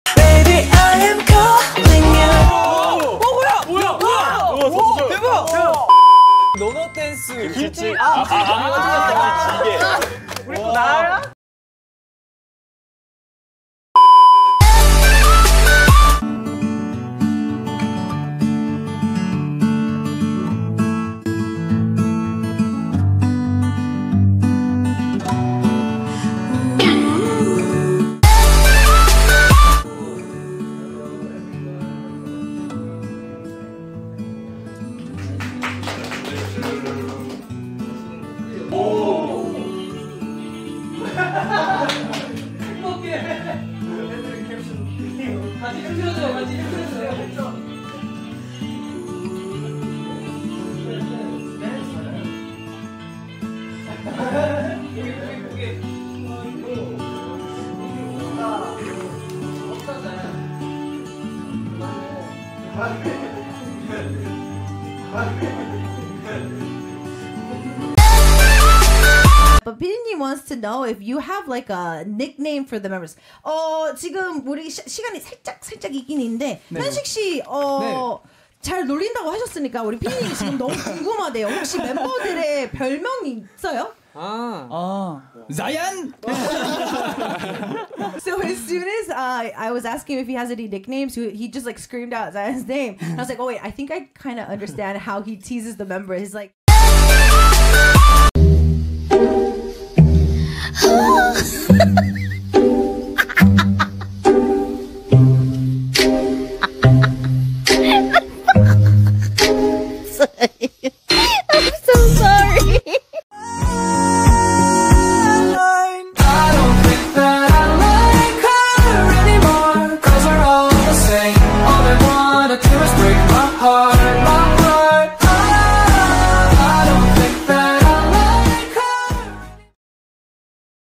Hyunsik and His First Child | Zayyan Hyunsik Moments
Sorry kalau ada beberapa kesalahan, beberapa member kepotong karena fokus video untuk Zayyan dan Hyunsik.